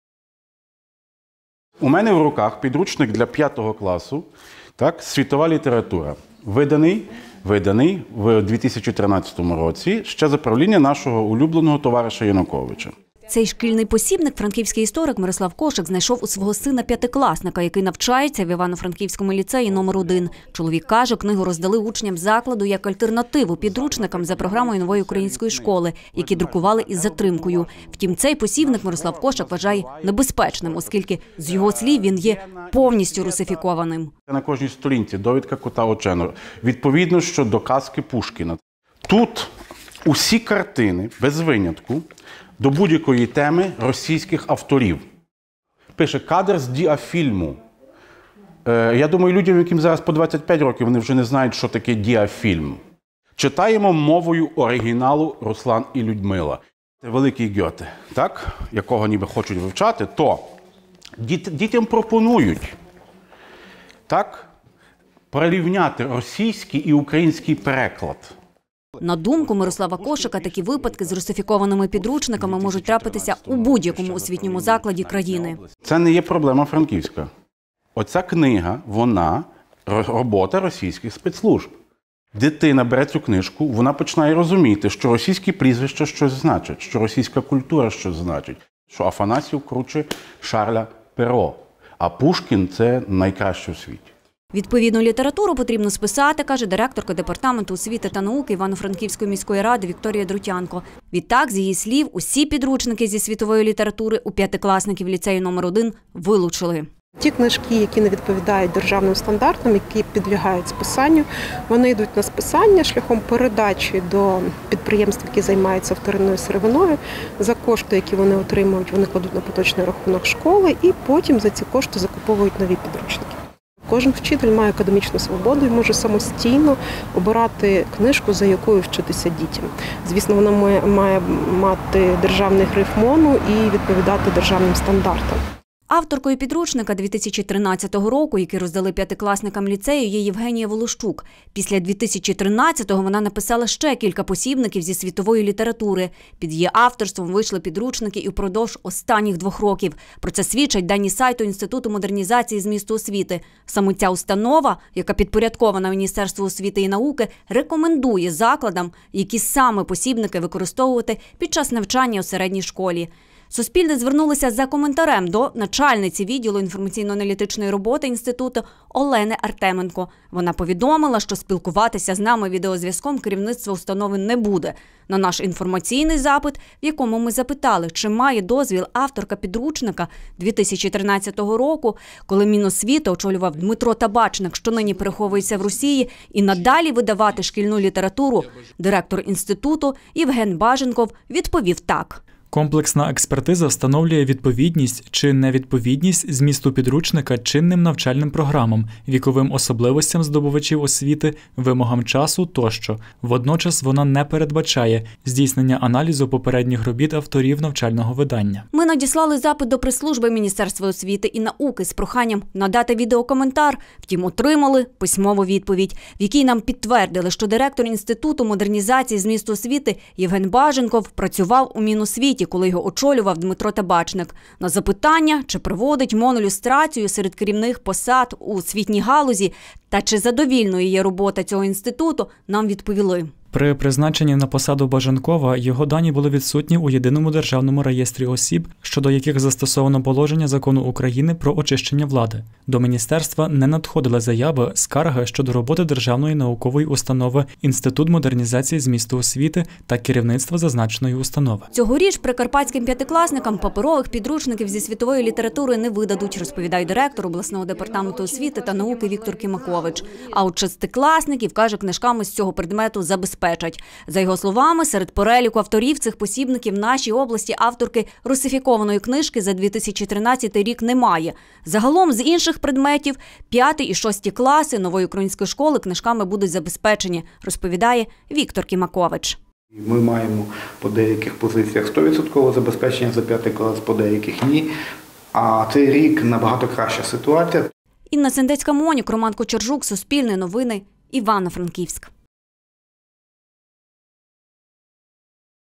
У ліцеї Франківська учням роздали підручники часів Табачника. Їхня авторка пише досі посібники
В одному з ліцеїв Івано-Франківська п'ятикласникам видали підручники зі Світової літератури за 2013 рік, орієнтовані на російську літературу. Його написала Євгенія Волощук, яка продовжує досі видавати посібники для дітей. В Інституті модернізації змісту освіти при затвердженні офіційного грифа на шкільні підручники не аналізують попередні роботи їхніх авторів.